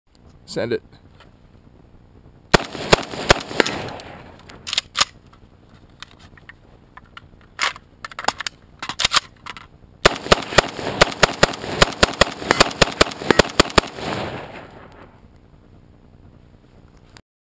VEPR SBR 1st RUN
We cut down a VEPR 5.45x39 to a 12.5" barrel and rethreaded for a standard AK74 brake. We've also modified this weapon with a left side charging handle to allow for faster mag changes. This is our first run trialing these mods. While I was slow on the mag change, I did speed up with the left side charging handle and didn't have to reach over or rotate the gun, thereby getting my weapon back in the fight sooner. Watch the recoil mitigation. this weapon shoots like a dream. Want your VEPR modded?